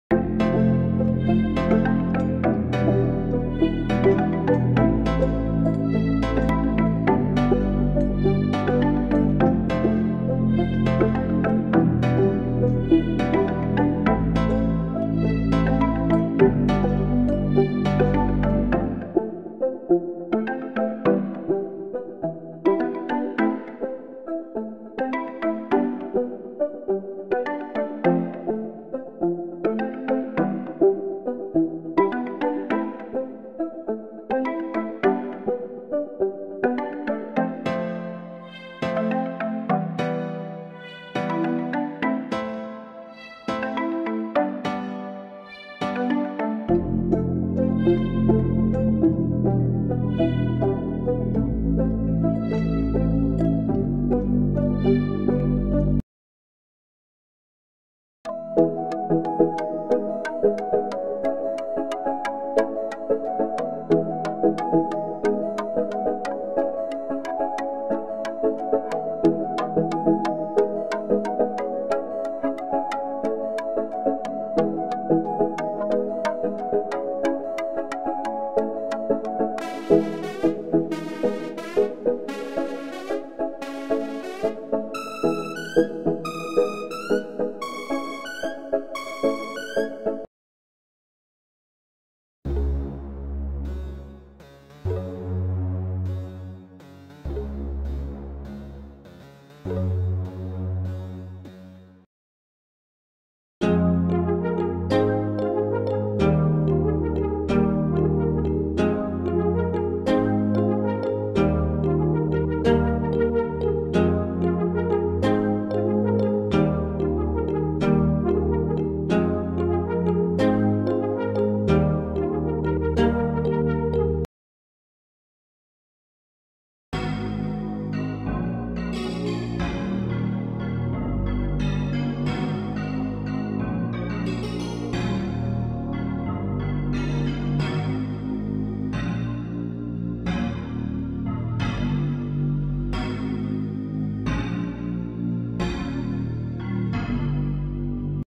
[FREE] Dancehall Afro loop kit - 2022
✅ Follow on IG - @Ayowdelus

~ This loop kit is inspired by dancehall music around the world.
The loop consist of 5 loops with stems and also key label with BPM of the loop.
~ My tag must be Included in your beat for use of these loops.
Any Placement with major artist must be cleared with me or copyright actions will be taken.

0:00 - 0:00
0:00 - 0:55 Loop 1
0:58 - 1:30 Loop 2
1:32 - 1:42 Loop 3
1:43 -2:04 Loop 4
2:04 - 2:29 Loop 5